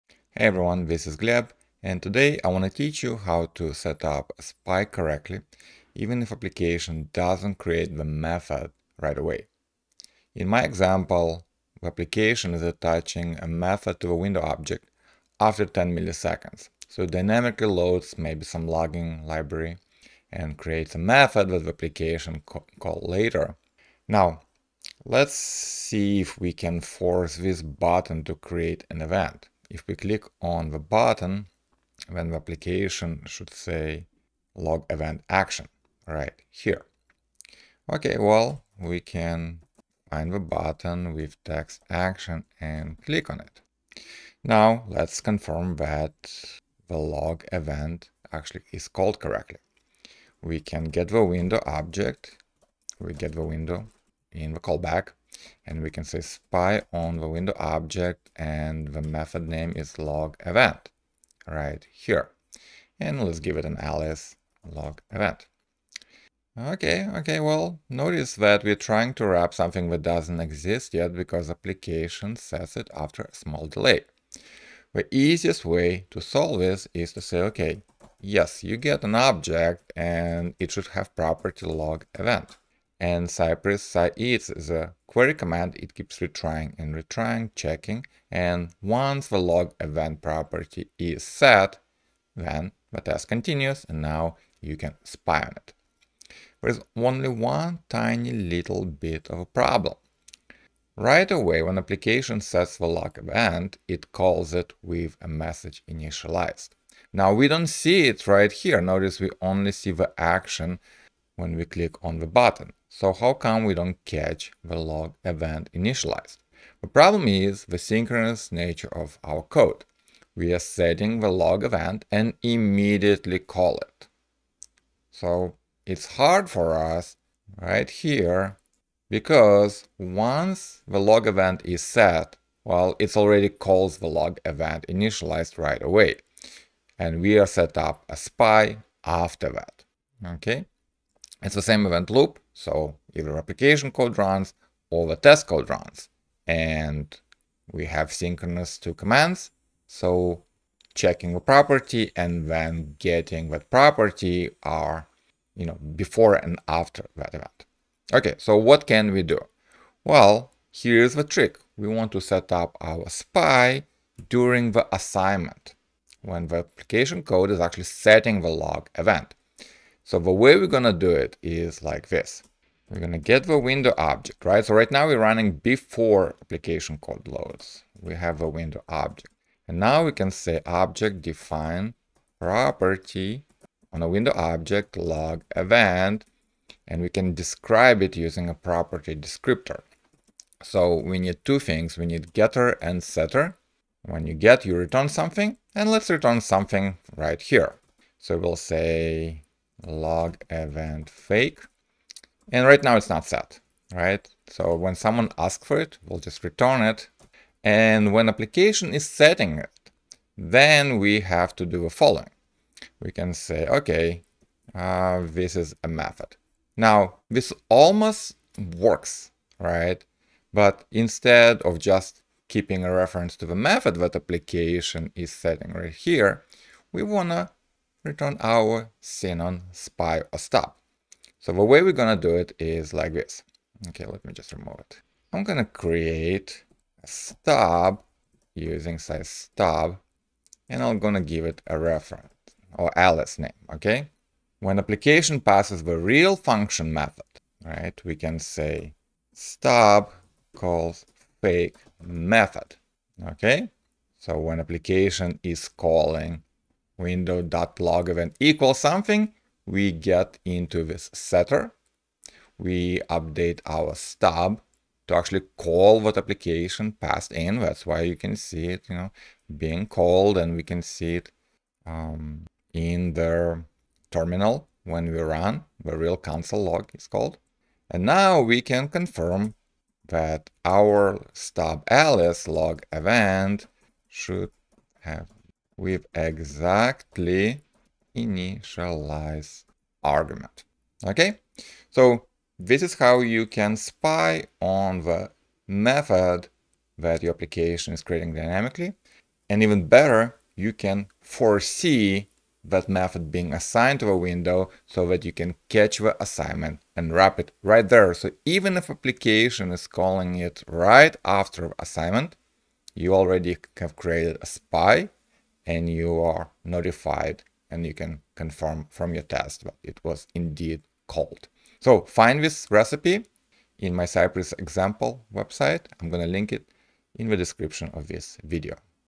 Prepare To Spy On A Method Added Dynamically
This video teaches you two things:
- how to wait for the method to exist before spying on it
- how to start spying on the method _immediately_ when the application creates it.